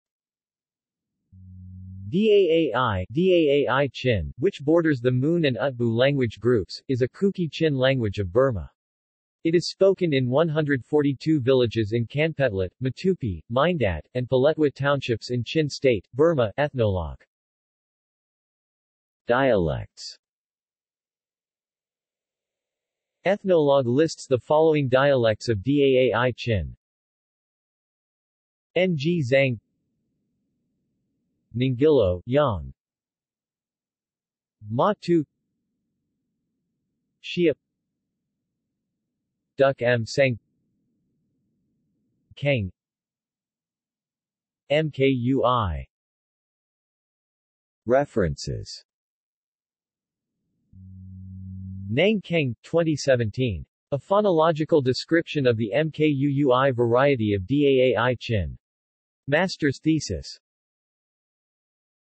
Daai language | Wikipedia audio article
This is an audio version of the Wikipedia Article:
Daai language

SUMMARY
Daai (Daai Chin), which borders the Mün and Ütbü language groups, is a Kuki-Chin language of Burma. It is spoken in 142 villages in Kanpetlet, Matupi, Mindat, and Paletwa townships in Chin State, Burma (Ethnologue).